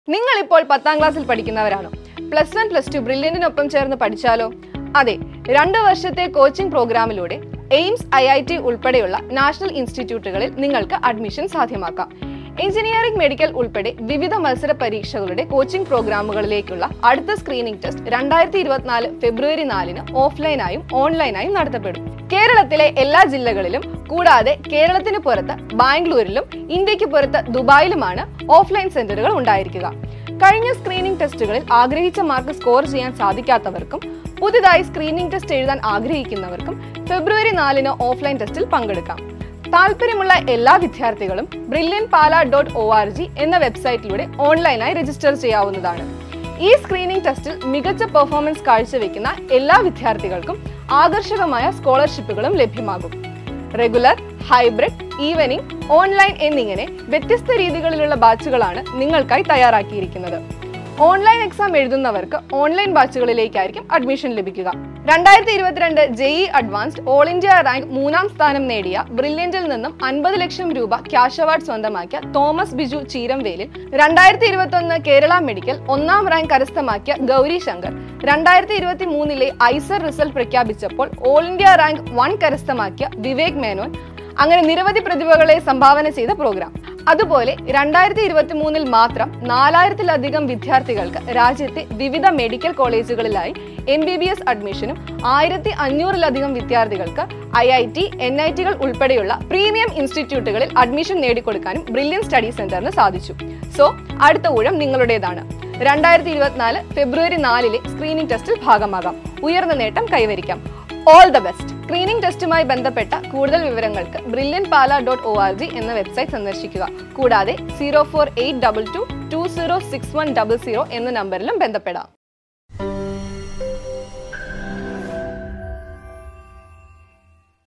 Here is an opportunity for students who are studying in class 10 | Register Now !
IIT/AIIMS Integrated batch is a two-year programme for the students of classes +1 and +2 to prepare for the entrance exams. Through this integrated course we aim to provide the best entrance coaching to get admission at the premium institutes in India such as AIIMS, AFMC, JIPMER, IIT, NIT, IISER, IIST, BITS and other national institutes. In this video, we have included the success stories of the toppers from the IIT/AIIMS Integrated batch.